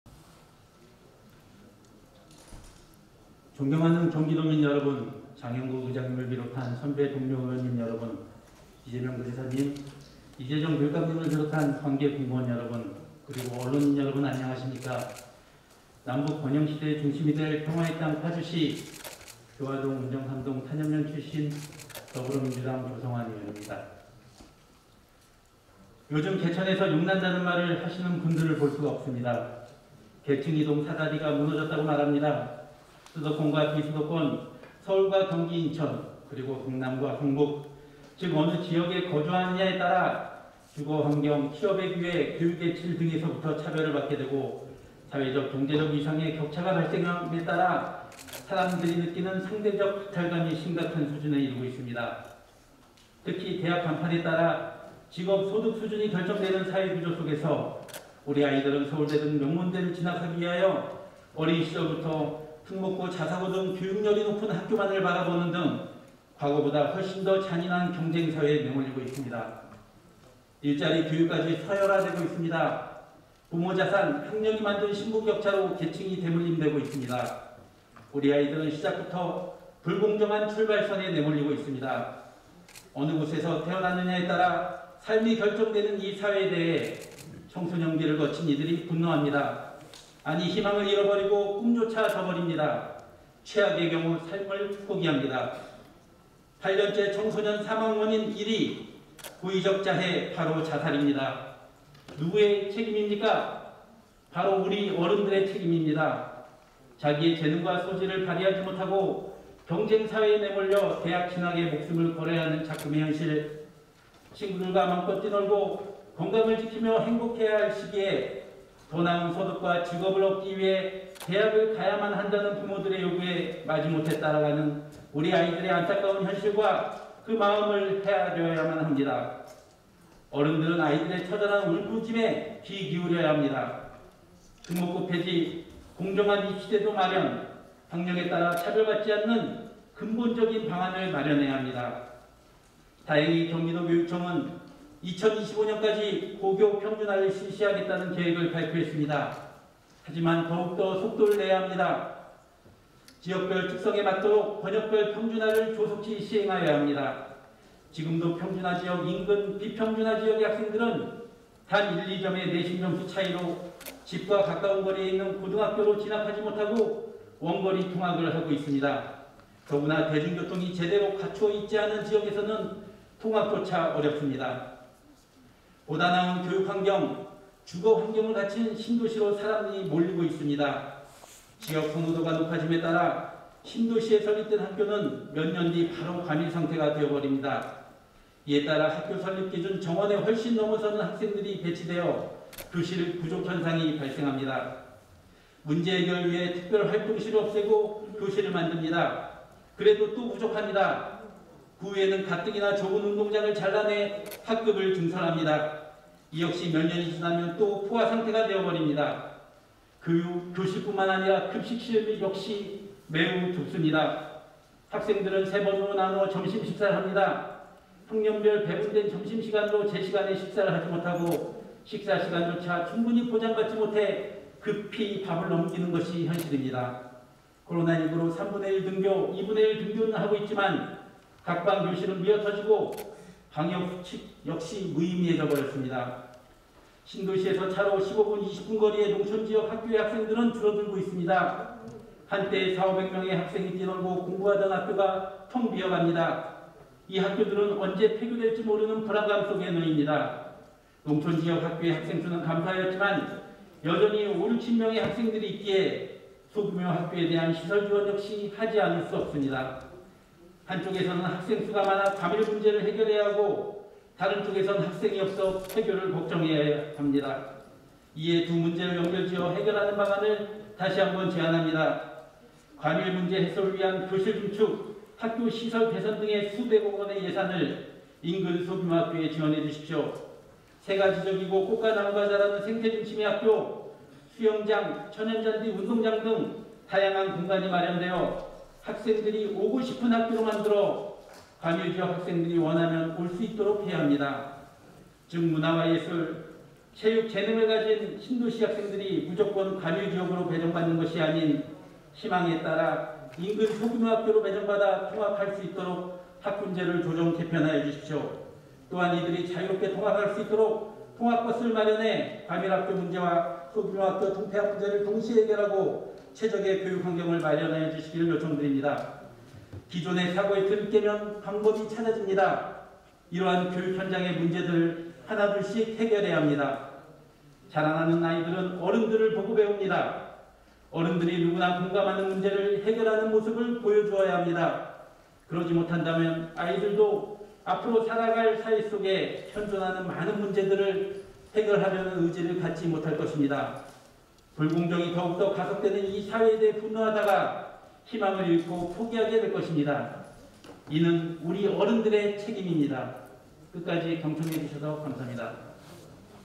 경기도의회 조성환의원 5분자유발언
[양해용기자]=경기도의회 여성가족평생교육위원회 조성환 의원(더불어민주당, 파주1)은 29일(목) 제351회 임시회 제4차 본회의에서 ‘불공정에 놓인 교육제도’에 대하여 질타하고, 해결방안을 마련할 것을 촉구하였다.

조성환 의원은 이날 5분 자유발언에서 “대학 간판에 따라 직업, 소득수준이 결정되는 사회구조 속에서 우리 아이들은 ‘서울대’ 등 명문대를 진학하기 위하여 어린 시절부터 특목고, 자사고 등 교육열이 높은 학교만을 바라보는 등 과거보다 훨씬 더 잔인한 경쟁 사회에 내몰리고 있다”라며 “경쟁사회에 내몰려 대학 진학에 목숨을 걸어야 하는 작금의 현실 속에서 우리는 근본적 교육제도 개선 방안을 마련해야한다”고 질타했다.

이어 “아직도 비평준화 학생들은 단 1~2점의 내신점수 차이로, 집 가까이 있는 학교로 진학하지 못하고 원거리 통학을 해야하는 것이 현실”이라고 안타까움을 표하며 “경기도교육청은 2025년 고교평준화를 실시하겠다고 하였으나, 지역별 특성에 맞는 권역별 평준화의 속도를 높여 고교평준화를 조속히 시행할 수 있는 방안을 마련해야한다”고 강조했다.

또한 “보다나은 교육환경과 주거환경을 위하여 신도시로 인구가 집중됨에 따라, 신도시에 있는 학급들은 과밀이 되고 농촌지역ㆍ구도심의 학급들은 학생 수가 없어 폐교 위기에 놓여있다”며 “과밀학교 문제 해소를 위해 단순히 교실을 지속적으로 증축하는 것이 아닌 과밀과 과소 학급의 두 문제를 연결하여 해결할 수 있는 방안을 고민해야한다”고 강력하게 주장했다.

이어 “신도시 학생들이 단순히 과밀지역으로 밀집되는 것이 아닌 인근 좋은 소규모 학교로 통학할 수 있도록 학군제를 개편하고, 인근 소규모 학교에 수영장, 천연잔디 운동장, 생태 환경 구축 등의 공간을 구축하여 학생들이 오고 싶은 학교로 만들어야 한다”며 “또한 원거리로 이동하는 학생을 위하여 통학버스를 운행하여 자유로운 등하교 방안을 마련한다면, 두 문제를 해결하는 최적의 교육환경이 조성될 것이다”고 제안했다.

마지막으로 조 의원은 “기존 사고의 틀을 깨면 방법이 찾아진다”라며 “불공정이 가속화되는 현 사회에 대한 분노로 우리 아이들이 자신의 꿈을 포기하는 일이 발생하지 않도록 어른들이 이를 책임지고 깊이 생각하여, 아이들이 살아갈 사회 속에 현존하는 많은 문제의 해결방안을 조속히 마련하길 희망한다”며 발언을 마무리 하였다.